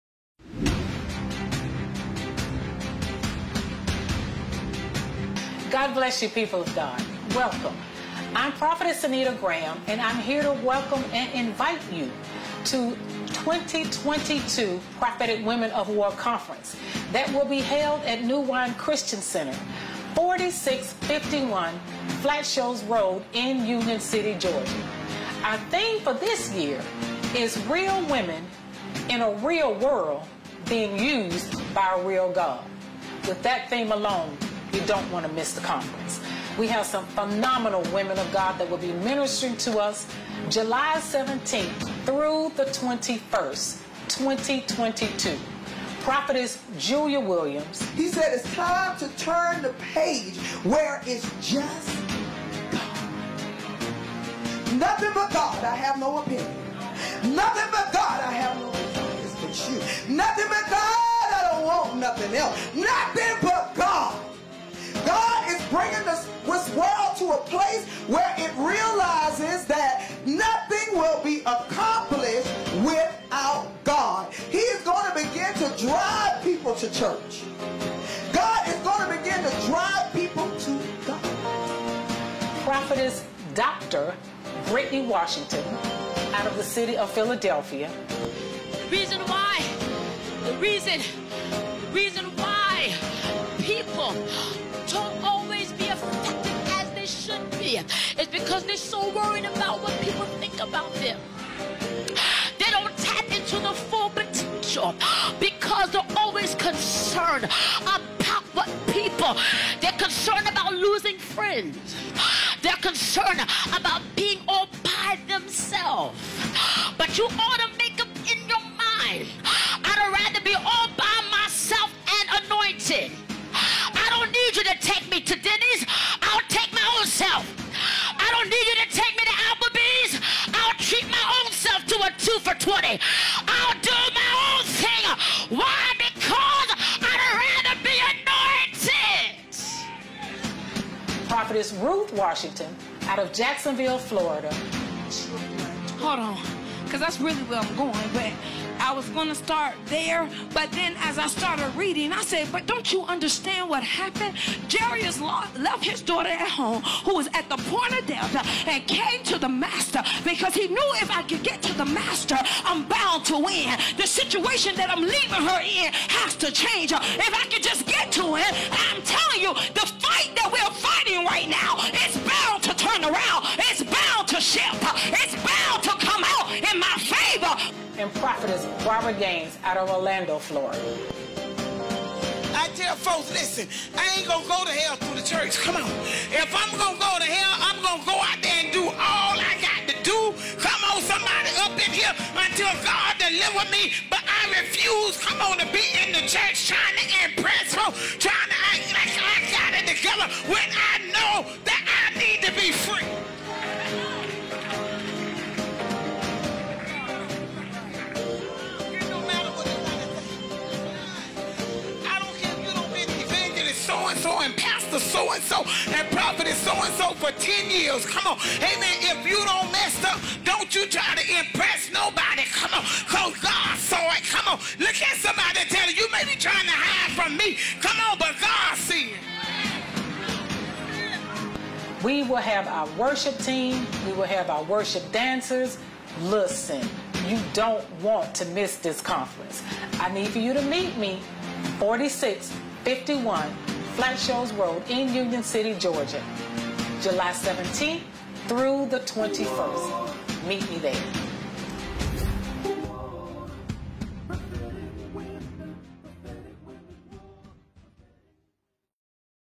2022 PWOW Conference Promo
Join us July 17th – 21st, for five powerful days of morning teaching sessions and evening worship services along with special guest speakers, music, worship dancing, and more. Come experience the life changing power of God!

Service Times:

10:00 AM – Prophetess Anita Graham (Conference Host)
7:00 PM – Prophetess Julia Williams

Monday, July 18th
9:00 AM – Evangelist Jameela Pruitt
7:00 PM – Prophetess Chastity Bradford

Tuesday, July 19th
9:00 AM – Evangelist Adrienne Dunston
7:00 PM – Prophetess Ruth Washington

Wednesday, July 20th
9:00 AM – Evangelist Cornisa Alston
7:00 PM – Dr. Brittany Washington

Thursday, July 21st
9:00 AM – Prophetess Akia Powell
7:00 PM – Prophetess Barbara Gaines

Due to COVID, seating is limited 75 adults per service and will be on a first come first serve basis.
Watch the conference online LIVE on Facebook, YouTube, or Boxcast.